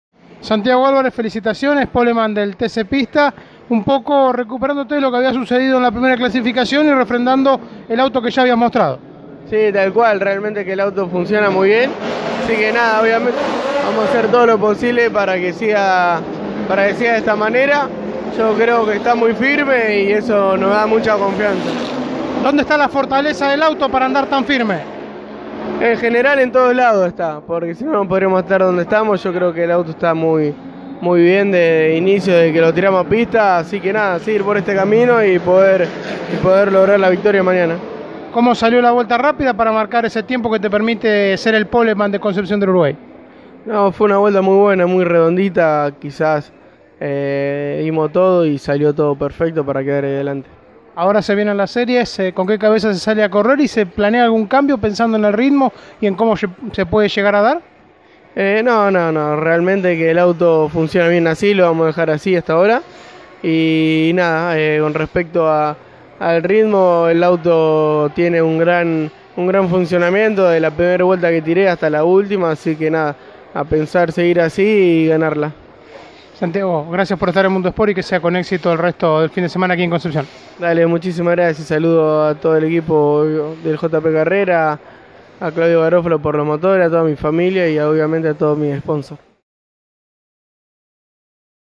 Santiago Alvarez - Pole position TC CDU 2019
Luego de conseguir su primera pole position en el TC Pista en el autódromo de Concepción del Uruguay, Santiago Alvarez dejó sus conceptos en los micrófonos de Mundo Sport.